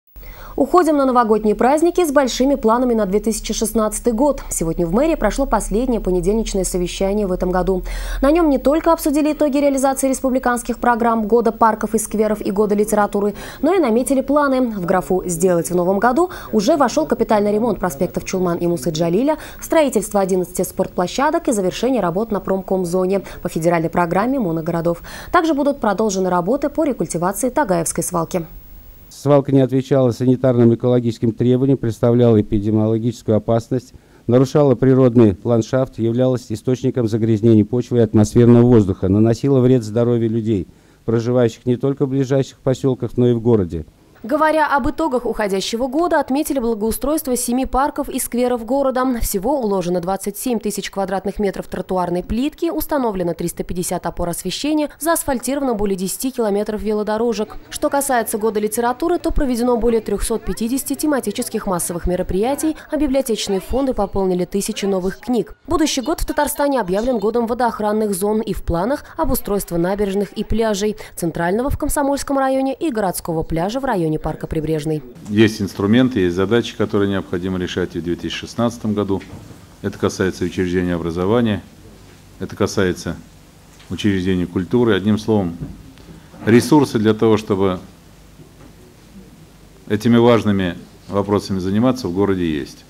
Подвели итоги, построили планы
Уходим на новогодние праздники с большими планами на 2016 год. Сегодня в мэрии прошло последнее понедельничное совещание в этом году. На нём не только обсудили итоги реализации республиканских программ Года парков и скверов и Года литературы, но и наметили планы.